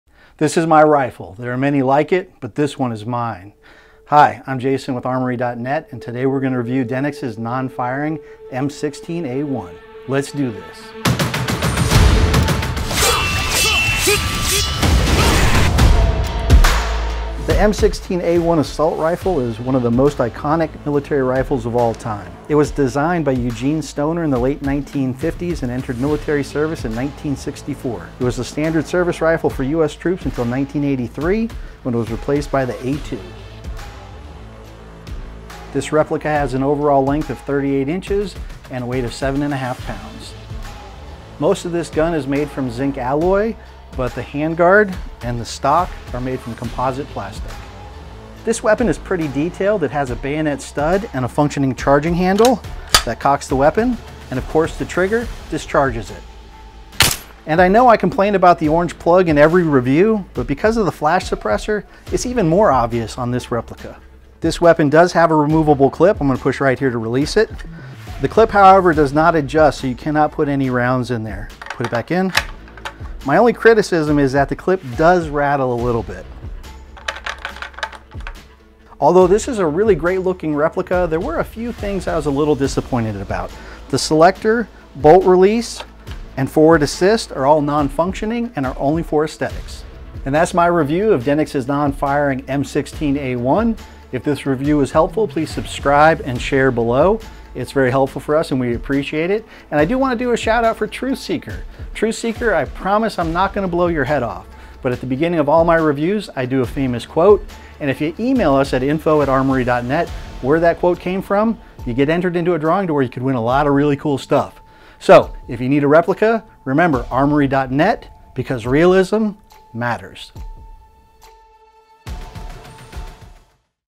DENIX M16A1 Assault Rifle Replica Review
The M16A1 was the standard military rifle from the early 1960's through the mid 1980's.  The M16A1 replaced the M14 and M1 Carbine and was later replaced by the M16A2.  Variations continue to be used today by military forces around the world.  A realistic look and solid build, combined with authentic moving parts.

Intro: (00:00)
M16A1 History: (00:17)
Length and Weight: (00:35)
Zinc and Plastic Parts: (00:41)
Charging Handle and Firing: (00:48)
Orange Plug: (00:57)
Removable Magazine: (01:04)
Non-Functioning Parts: (01:19)
Outro: (01:30)